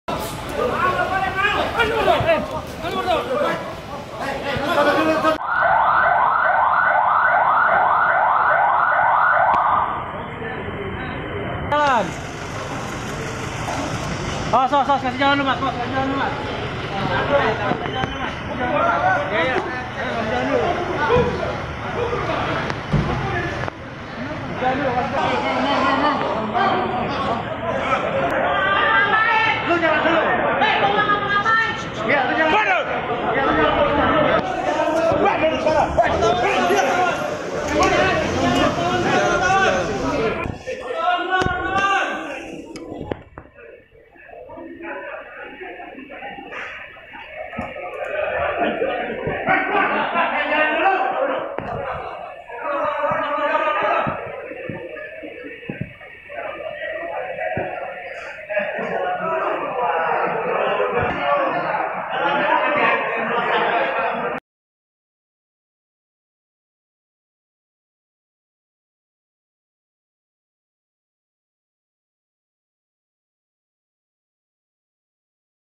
Video saat Hercules Rozario Ngamuk dan Kejar-kejar Wartawan
Hercules Rozario Marshal mengamuk saat turun dari mobil tahanan di lantai basement Pengadilan Negeri Jakarta Barat, Rabu, 27 Maret 2019.